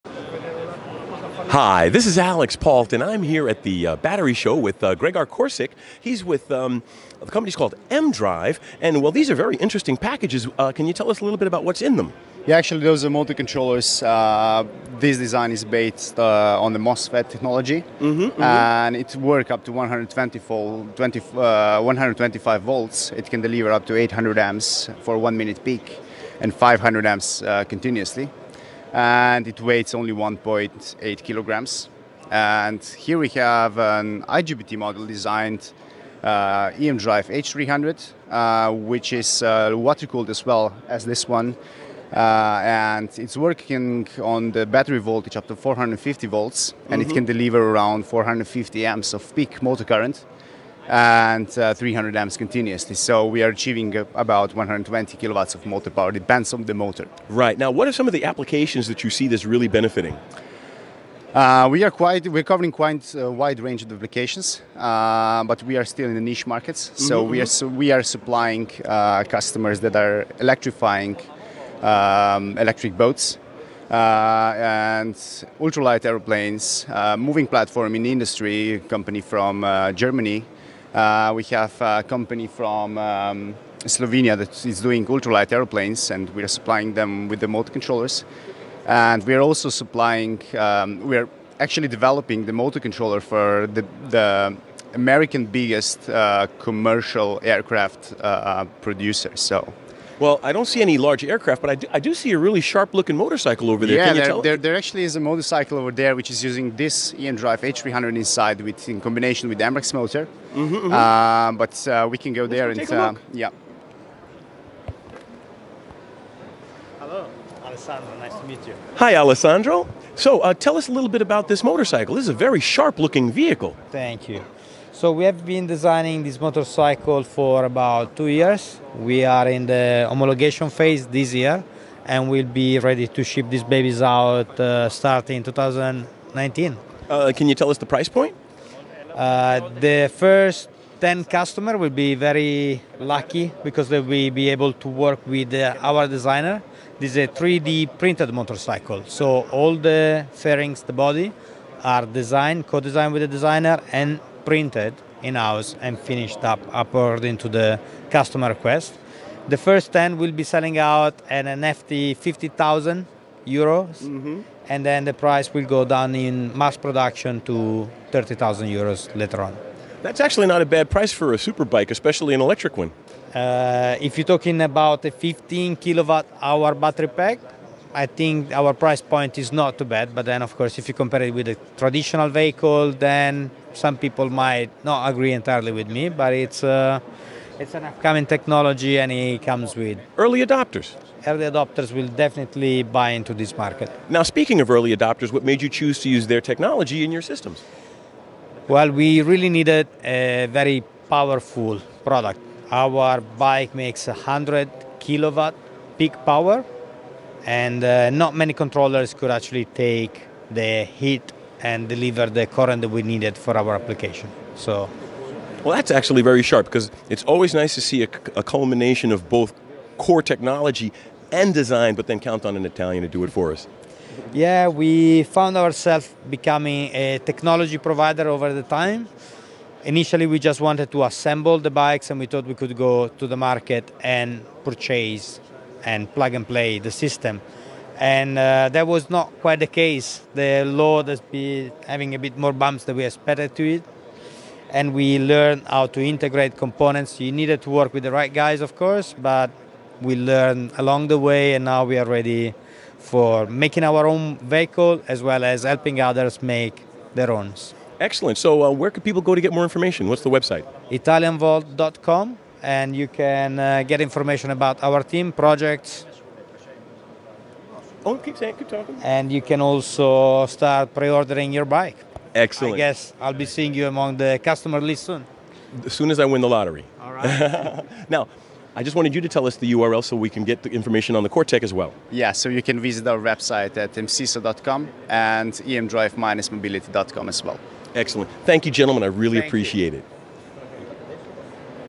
EMSISO talks about their latest motor controller technology
EMSISO had an electric superbike from Italian Volt at their exhibit at the Battery Show in Hannover, Germany. The bike is intended to highlight the company's emDrive technology, which uses a system to calculate the exact rotor angle, allowing more precise sinus motor current control. The end result is no motor vibration and improved torque.

The Lacama (inspired by the chameleon, as it is completely customizable for the user), as an acceleration from 0 to 100 Km/h (~60mph) in 4.6 seconds, with a range of approximately 200km using a battery with an energy Density of 114 Wh/kg (in capacities of up to 15Kwh). The maximum speed is limited to 180 Km/h, and there is also a parking mode that limits the bike to walking pace